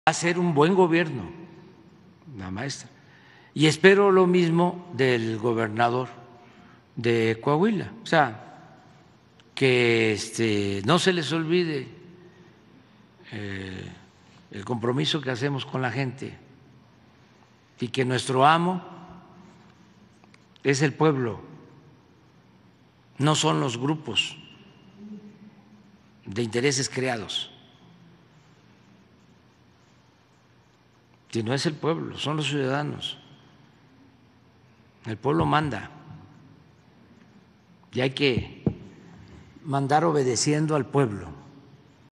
La maestra Delfina Gómez hará un buen gobierno y espero lo mismo en Coahuila: AMLO
La maestra Delfina Gómez hará un buen gobierno y espero lo mismo del gobernador de Coahuila. Que no se les olvide el compromiso es con la gente: AMLO, tras resultados del